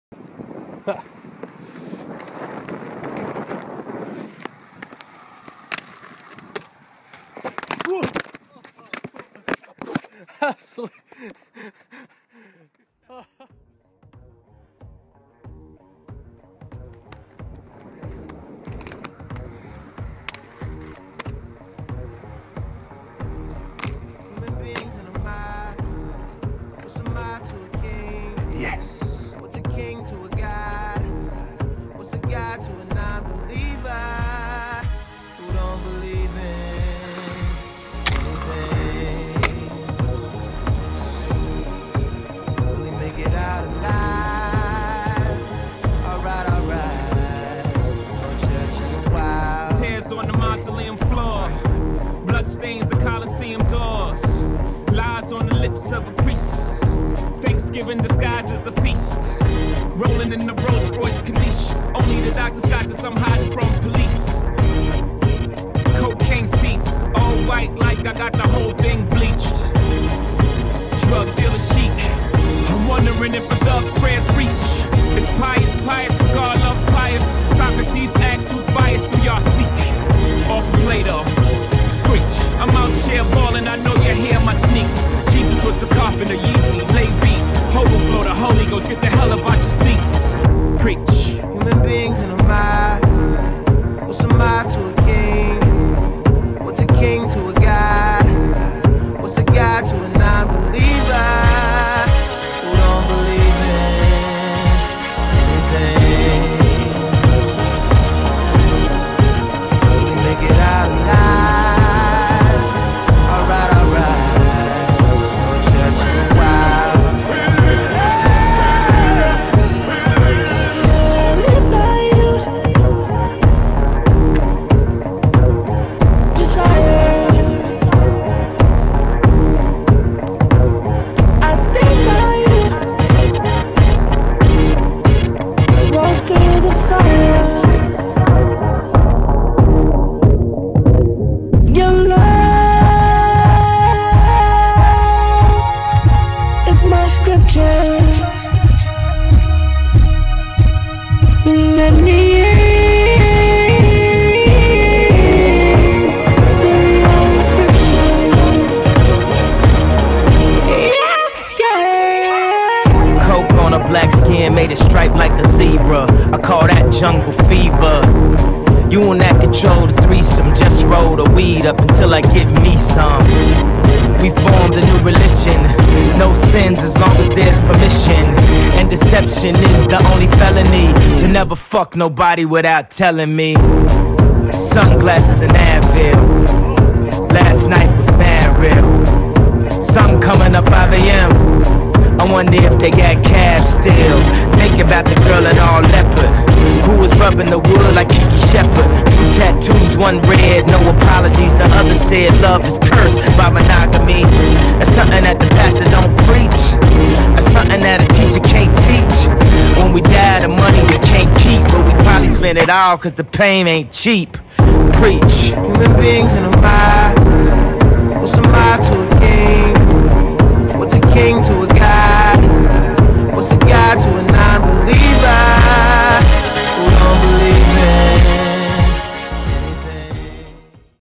Bob's iPhone edit
A few fun clips put together from one night down at the local park, enjoy!